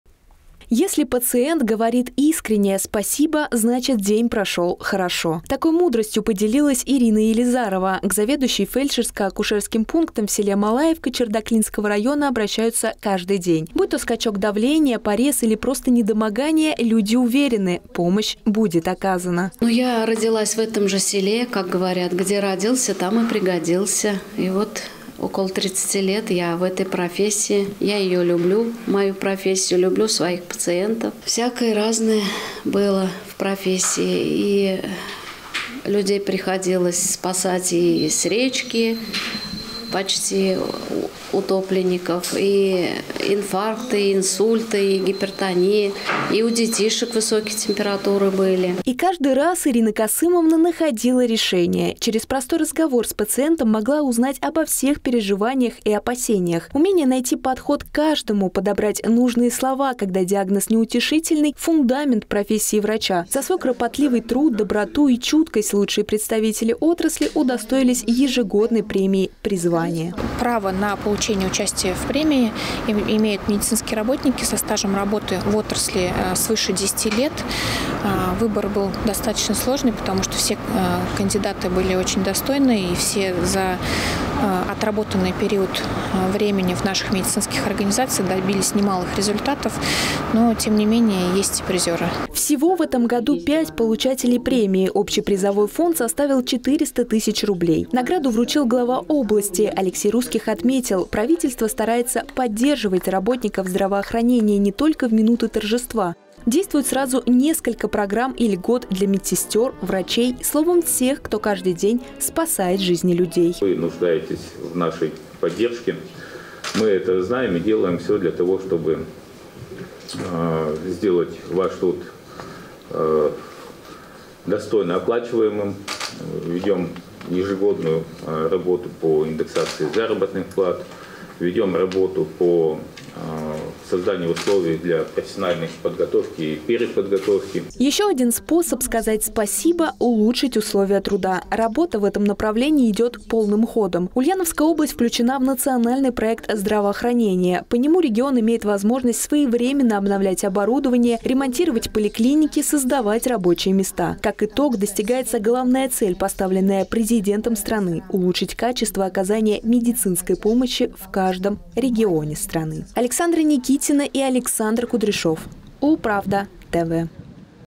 За верность профессии. Ульяновским медикам вручили премию «Призвание»
За усердный труд и доброту к людям. В Ульяновской области лучшие специалисты отрасли здравоохранения получили ежегодную премию «Призвание». Общий призовой фонд – 400 тысяч рублей. Однако главная награда для медиков – здоровые пациенты. Для этого медсестры, врачи и фельдшеры готовы трудиться днём и ночью.